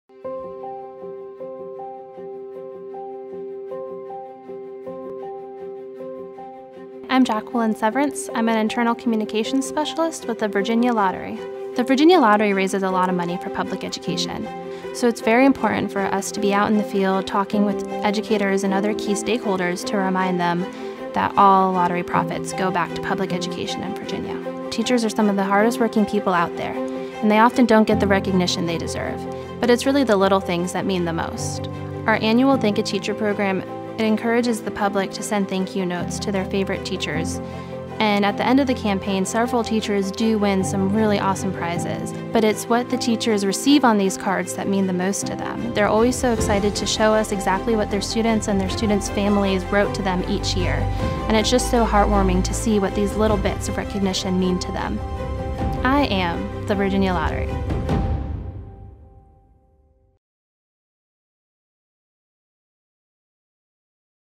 Get to know the Virginia Lottery | Mission
Interested in working at the Virginia Lottery? We've put together a series of videos to help share what it is about the Lottery that brings us together and why employees love being a part of the Virginia Lottery family!

Want to learn more?